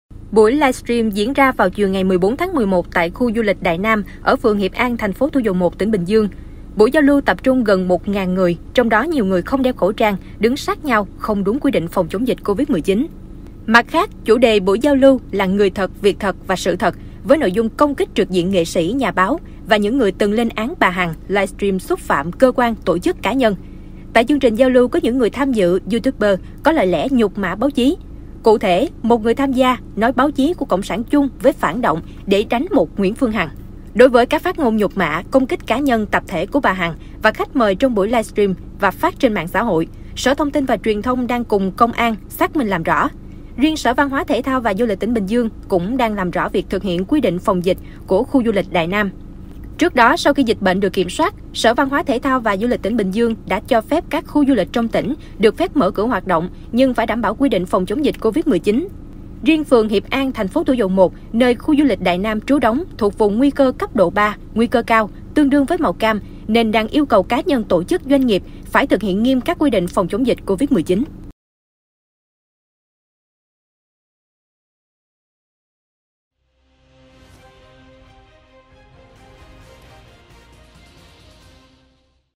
Làm rõ việc livestream của bà Nguyễn Phương Hằng tập trung đông người trái quy định
37 Bà Triệu, Hoàn Kiếm, Hà Nội, Việt Nam